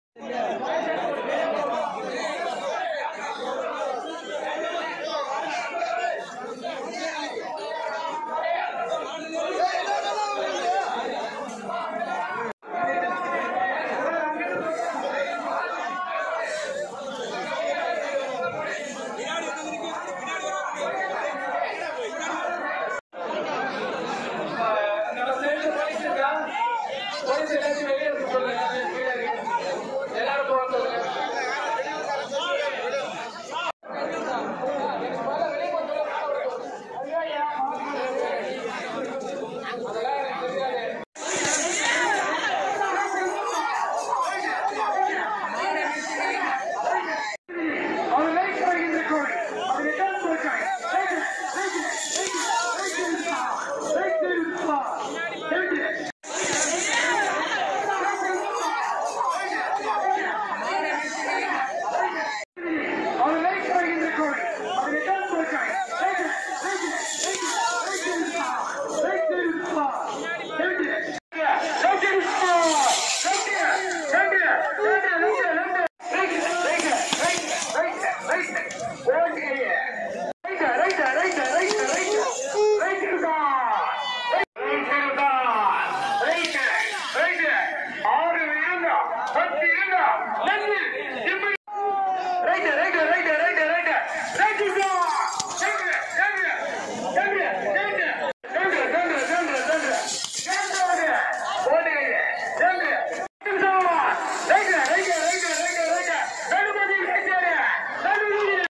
VLR PERNAMPET YERIKUTHTHI KALAIVIDUM, VIZHA
வேலூர்   19-1-23

பேர்ணாம்பட்டு அருகே ஏரிக்குத்திகிராமத்தில் காளைவிடும் விழாவானது நடைபெற்றது இதில் 150 காளைகள் பங்கேற்று ஓடியது
    வேலூர்மாவட்டம்,பேர்ணாம்பட்டு அருகேயுள்ள ஏரிக்குத்தி கிராமத்தில் காளைவிடும் திருவிழாவானது நடைபெற்றது இதில் பேர்ணாம்பட்டு குடியாத்தம்,கேவிக்குப்பம் ,காட்பாடி ஆந்திரா ஆகிய பகுதிகளிலிருந்து அழைத்து வரப்பட்ட 150 காளைகள் பங்கேற்று மின்னல் வேகாத்தில் சீறிப்பாய்ந்து ஓடியது இதனை ஆயிரக்கணக்கான பொதுமக்கள் கண்டு மகிழ்ந்தனர் இதில் 2 பேர் படுகாயமும் 29 பேருக்கு லேசான காயமும் ஏற்பட்டது காளைகள் வேகமாக ஓடி முதலிடத்தை பிடித்த காளையின் உரிமையாளருக்கு  முதல் பரிசாக ரூ.100001,இரண்டாம் பரிசு,ரூ.75 ஆயிரமும் ,மூன்றாம் பரிசு 65 ஆயிரம் உள்ளிட்ட 51 பரிசுகள் காளைகளின் உரிமையாளர்களுக்கு வழங்கப்பட்டது